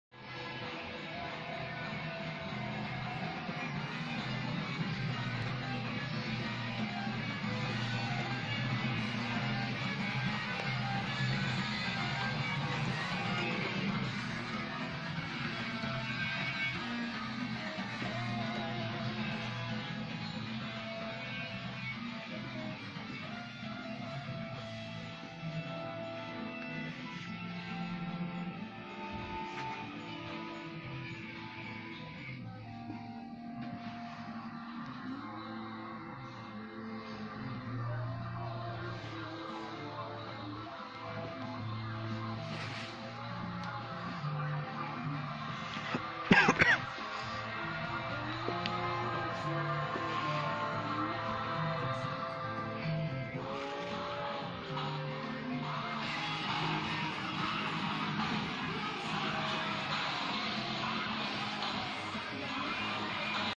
ikebe gakki drum station shinsaibashi, osaka ❤
hi gaiss, video ini diambil ketika aku first masuk ke ikebe gakki drum station, shinsaibashi.  ❤ terletak di bahagian americamura shinsaibashi.  best drum center ever in osaka ❤😁